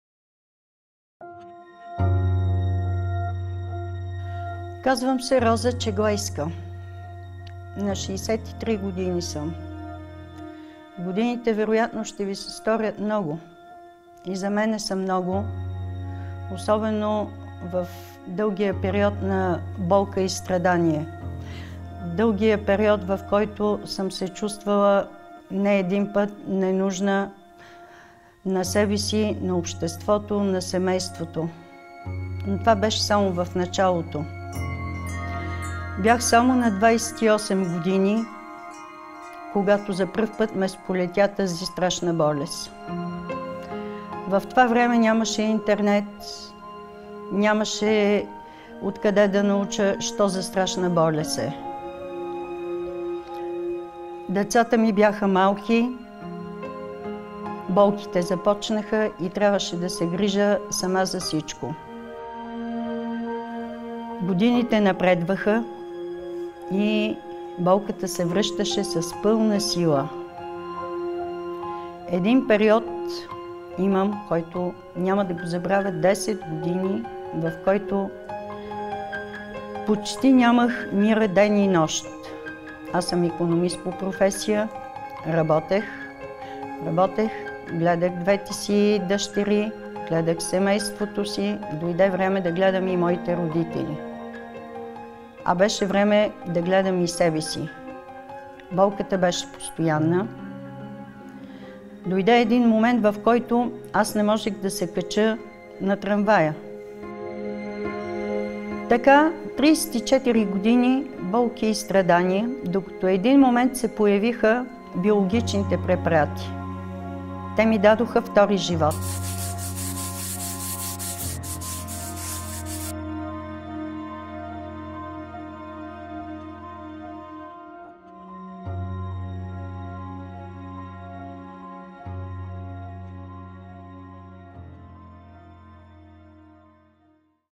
Роза е изчезващ вид - кампания
Искам да кажа на държавниците ни: Моля ви, не произвеждайте инвалиди! Прочетете повече за историята на Роза тук